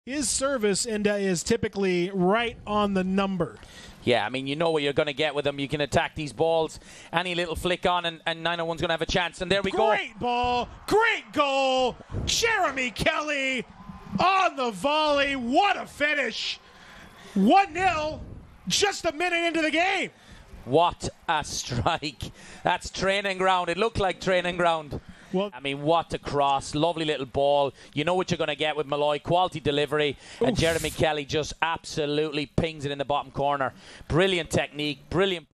Jeremy Kelly with a Goal vs. Miami FC
Jeremy Kelly (Memphis 901 FC) with a Goal vs. Miami FC, 06/03/2023